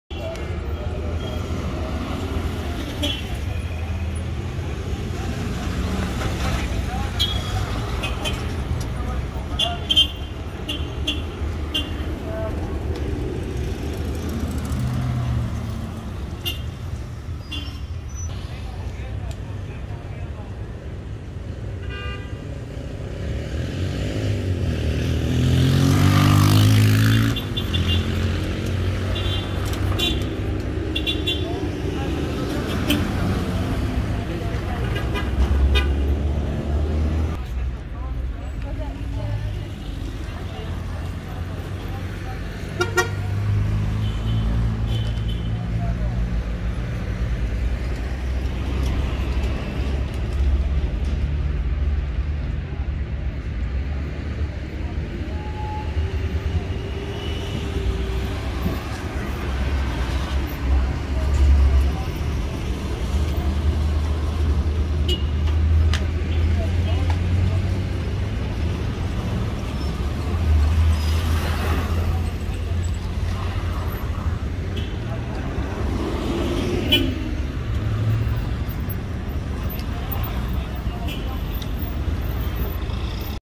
Malabarista en la Avenida Busch de La Paz
Ahora tenemos al Malabarista en la Avenida Busch en la ciudad de La Paz, que se gana unos pesos al dia